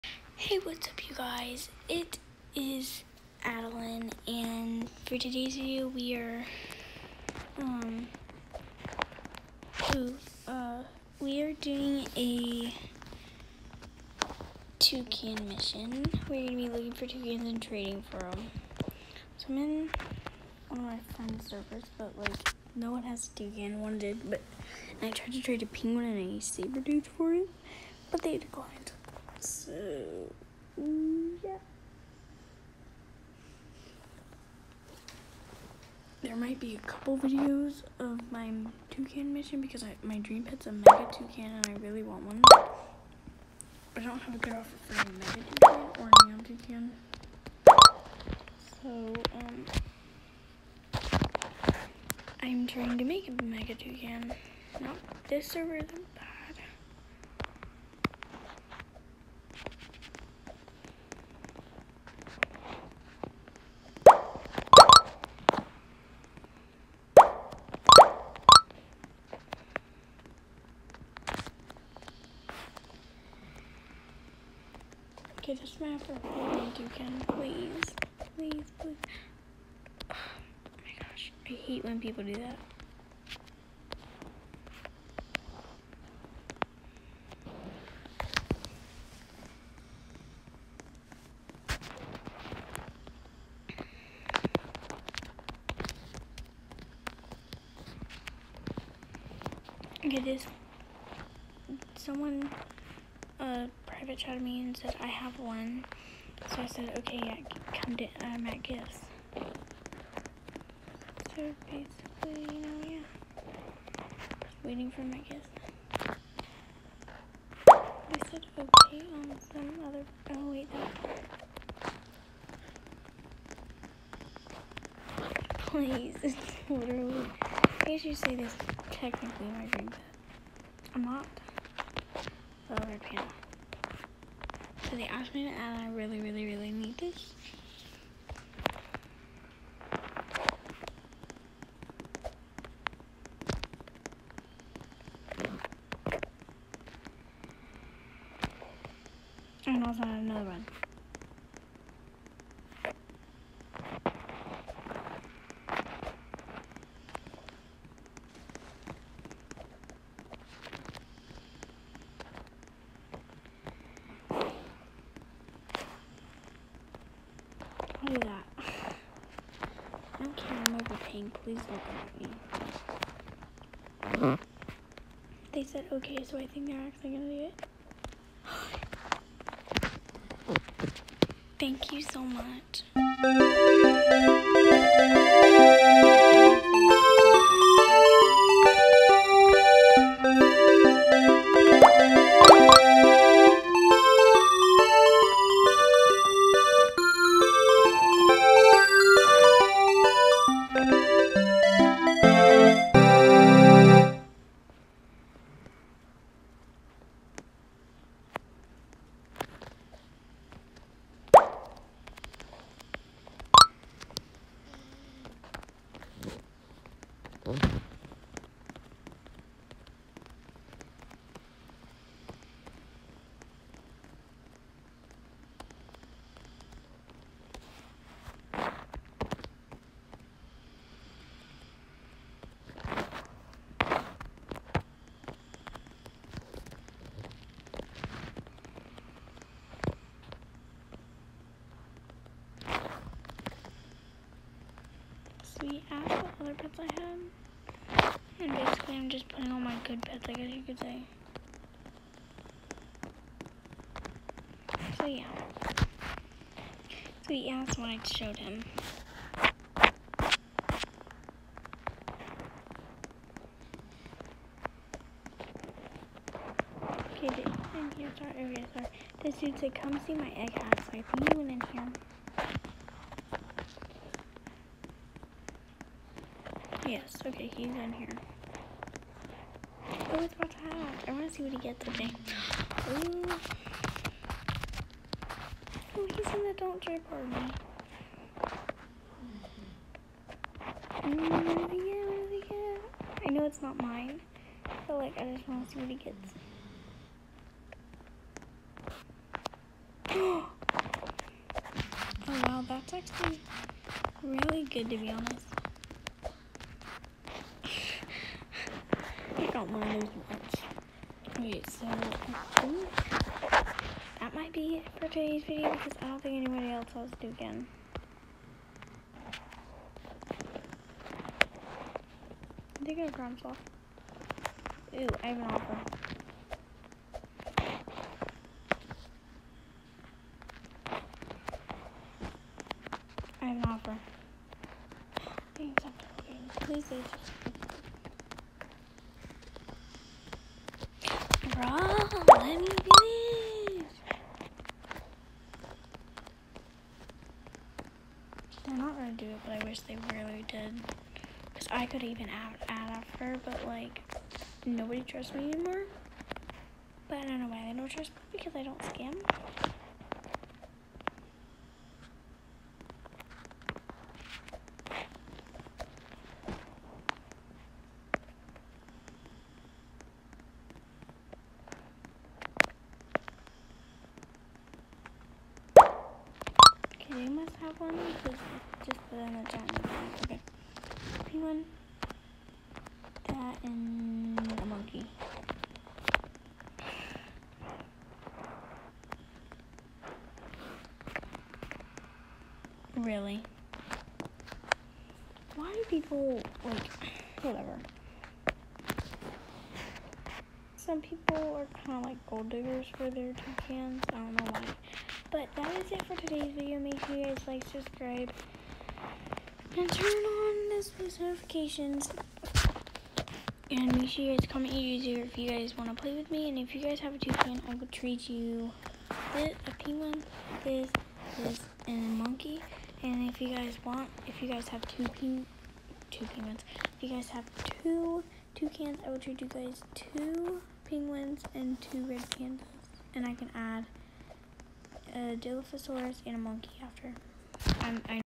Toucan mission!! |AddelinePlayz
Trying to make my dream pet mega toucan!!